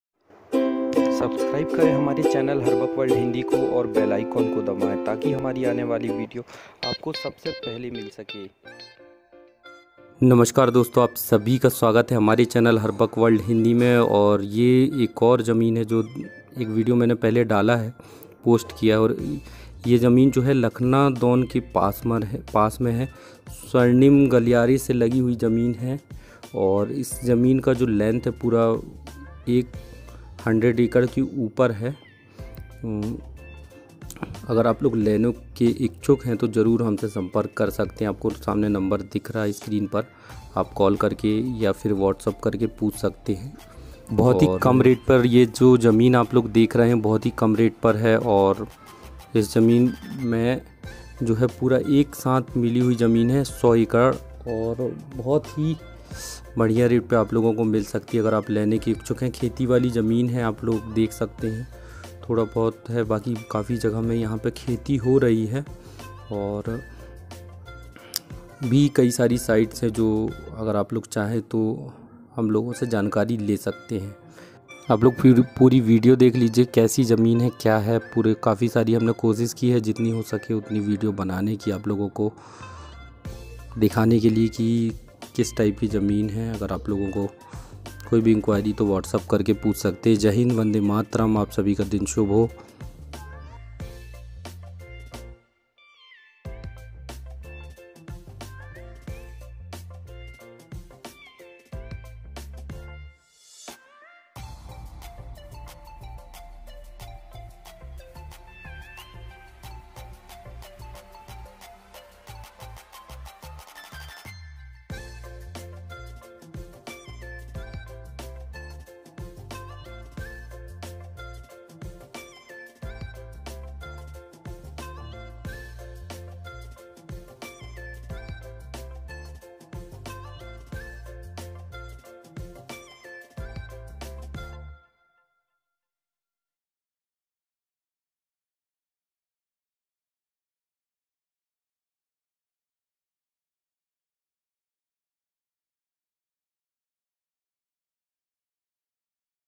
100 एकड़ जमीन खेती के लिए
100 एकड़ जमीन खेती के लिए चाहिए तो आप हम से संपर्क कर सकते है mandla sivni जिले में ये जमीन है whatup -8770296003